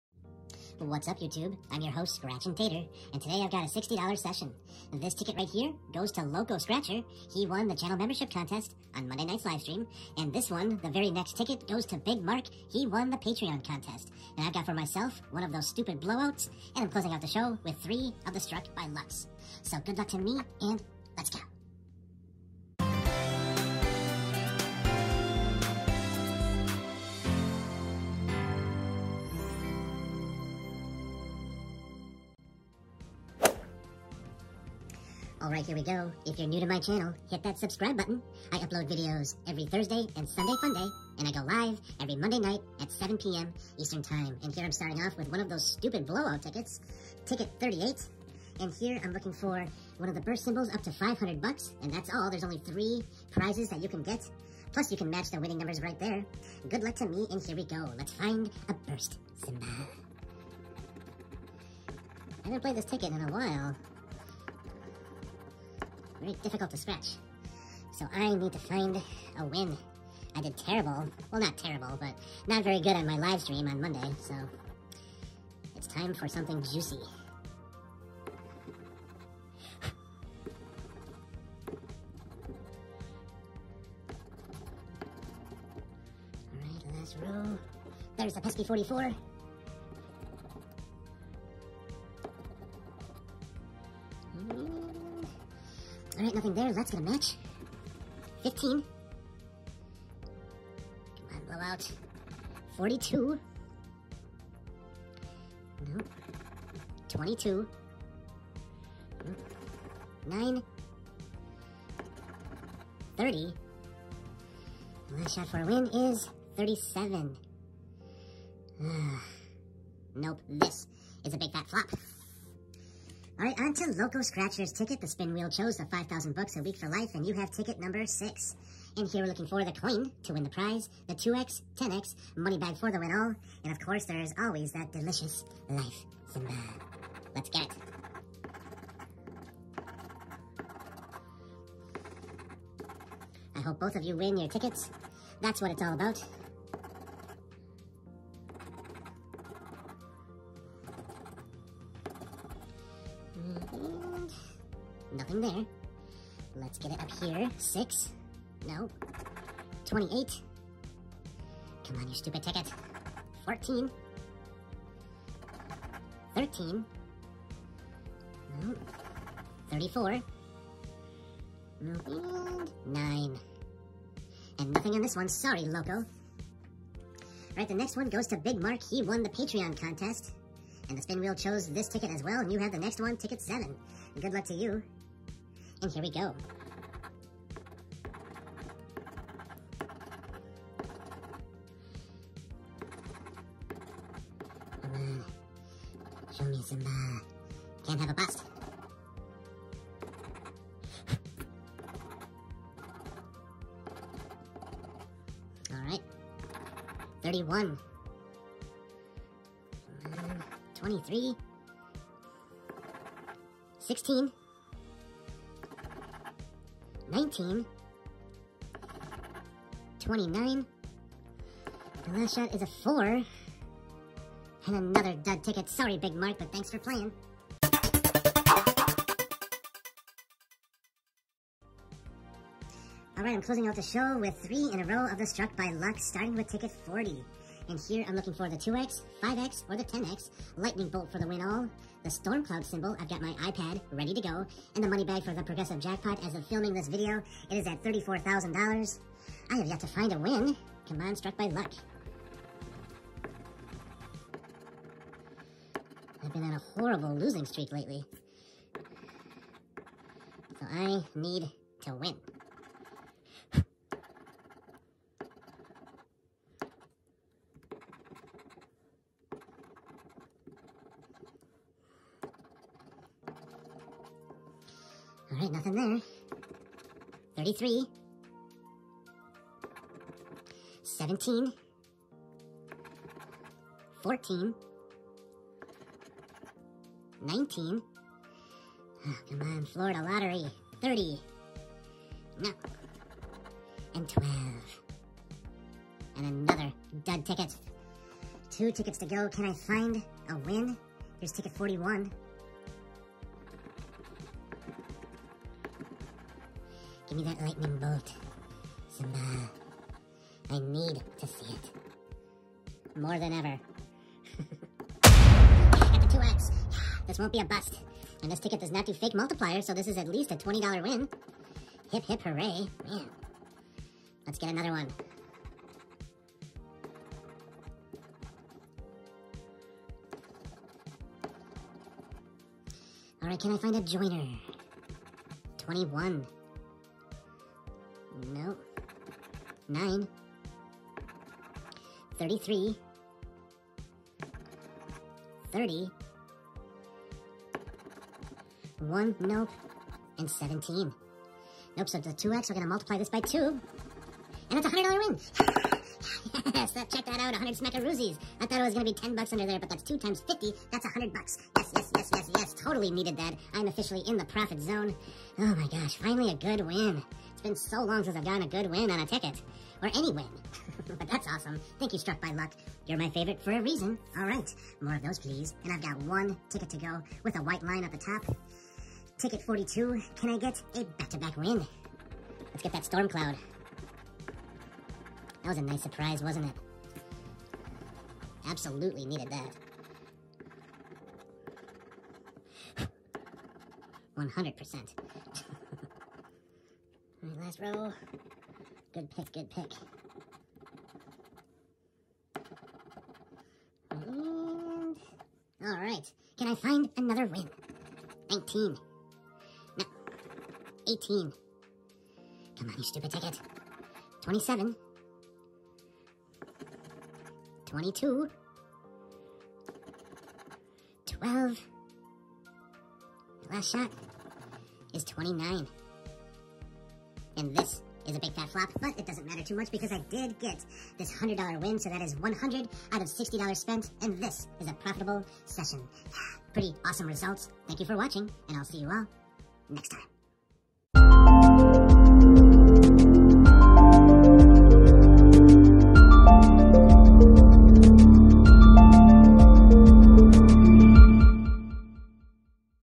NICE WIN ON THE STRUCK BY LUCK!!
Playing $60 worth of tickets from the Florida Lottery. Enjoy!

(2) $10 $5000 a Week For Life
(1) $10 Holiday Blowout
(3) $10 Struck by Luck

Send Spud Mail to:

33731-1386